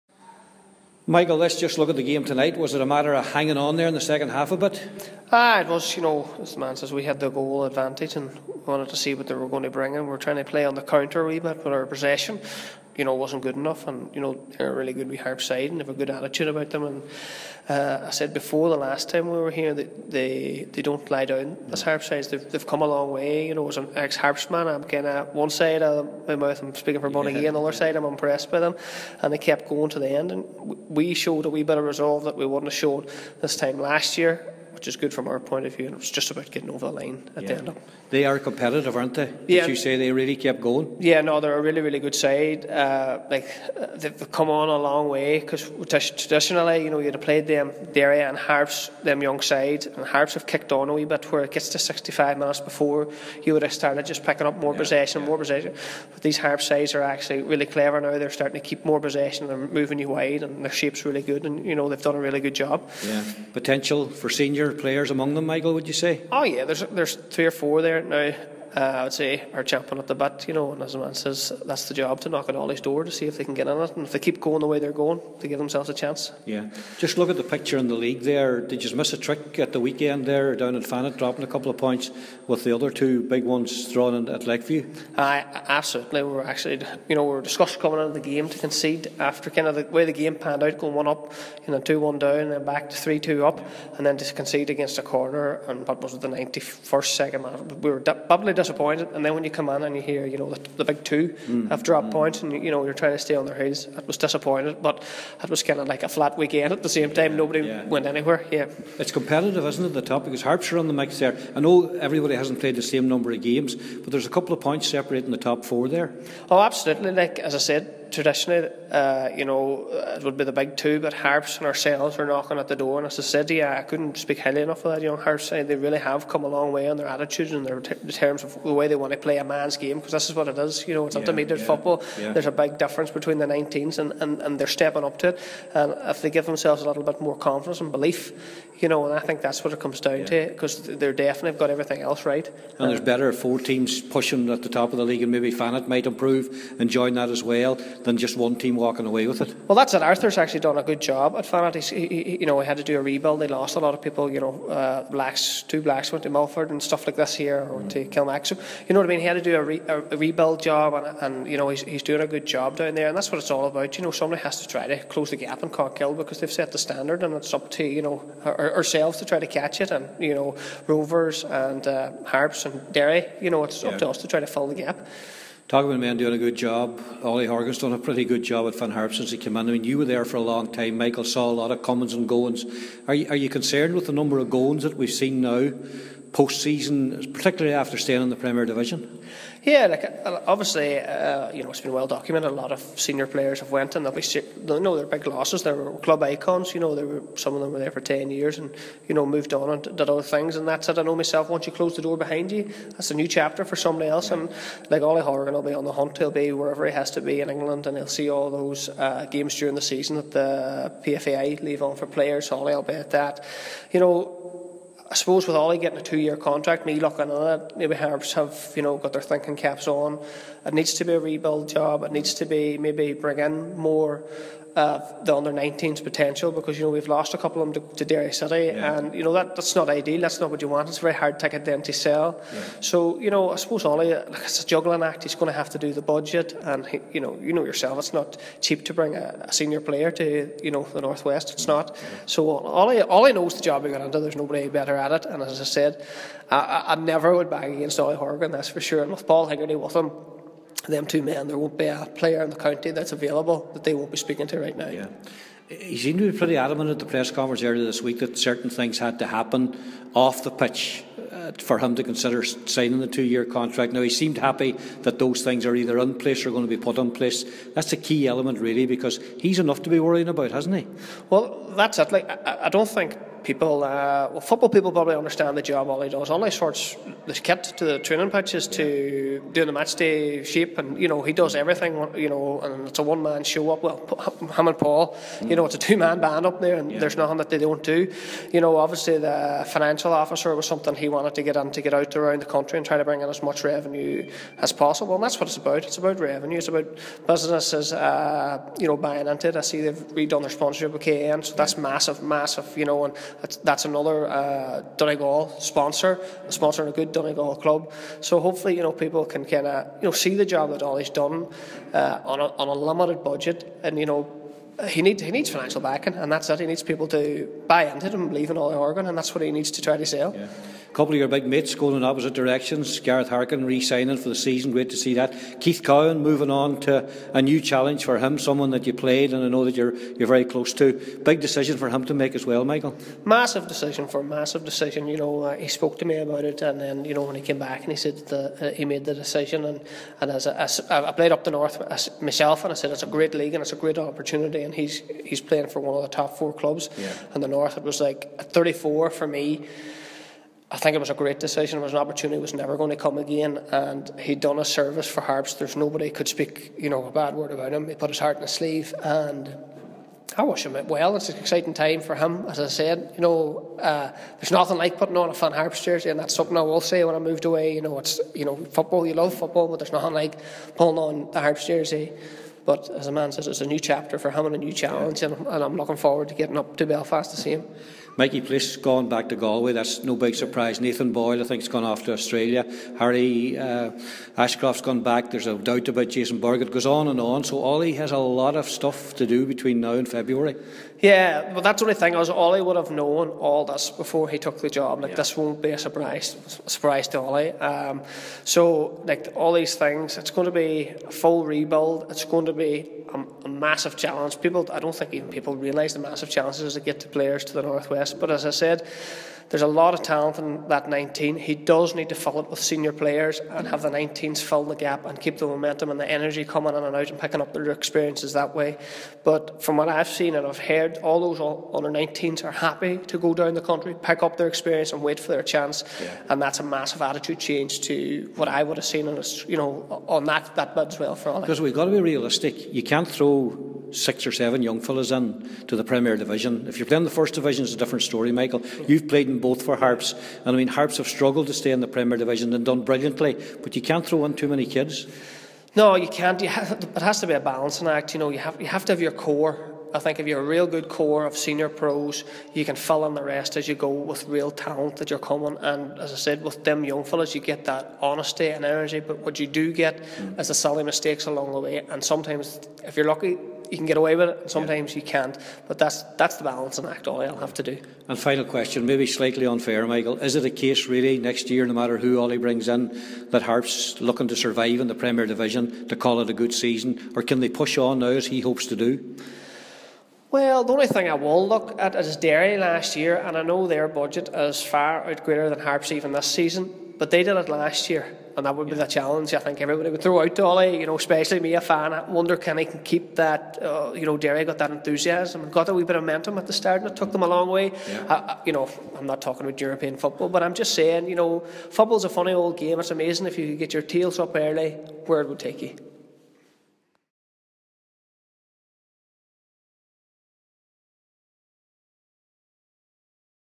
Michael Funston says Bonagee 'got over the line' against Finn Harps
Michael Funston played his part as Bonagee United booked their place in the semi-finals of the Knockalla Caravans Cup last night.

Bonagee defeated Finn Harps 1-0 with Michael Doherty scoring the only goal from the first half penalty. Funston, a former League of Ireland player with Harps, also gave his thoughts on where the Ballybofey club are headed in 2020.

He spoke to Charlie Collins at Dry Arch Park